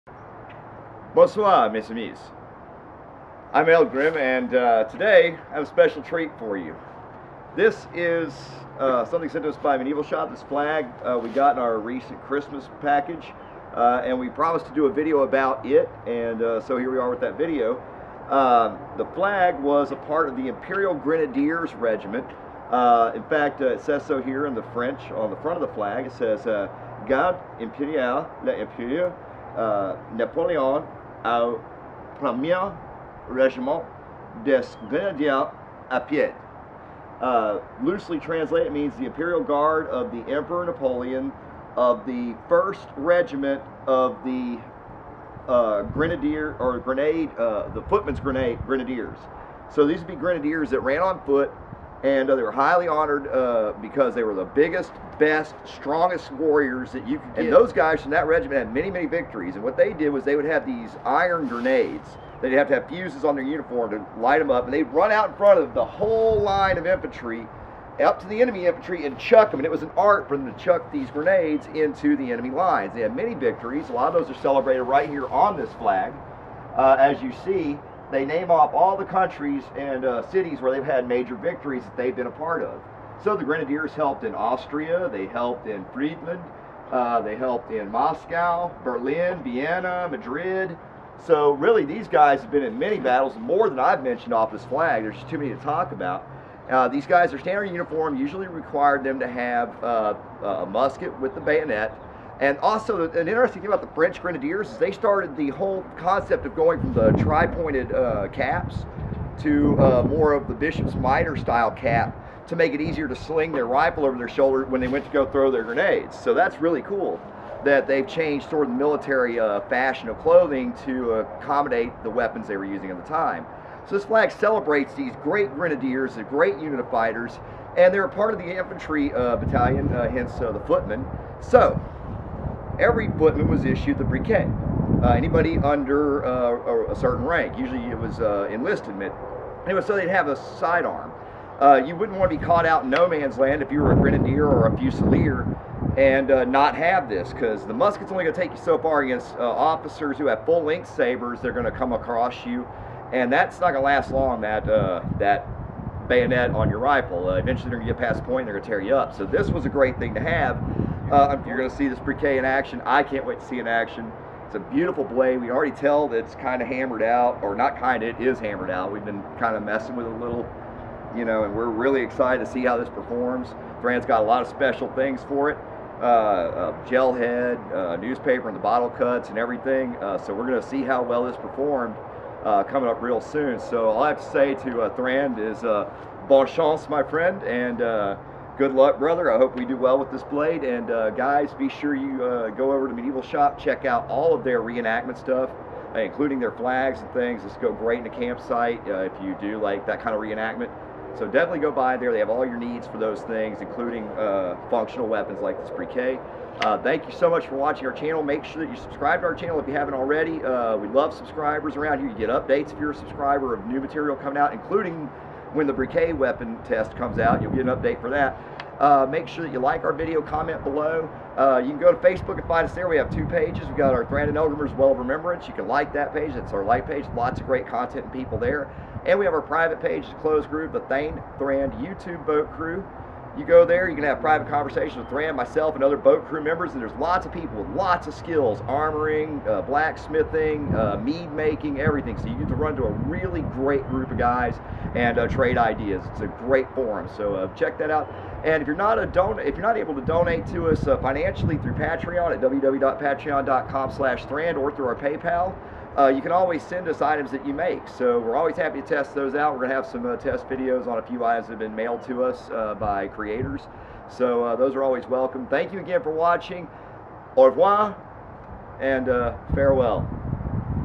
Napoleonic Battle Standard of the Imperial Guard Grenadiers!
Eldgrim Talks about the History of the Napoleonic battle standard of the Imperial Guard Grenadiers from Medieval Shoppe. So sit back grab an ale, mead or cognac and enjoy this fine battle standard of famed imperial Grenadiers!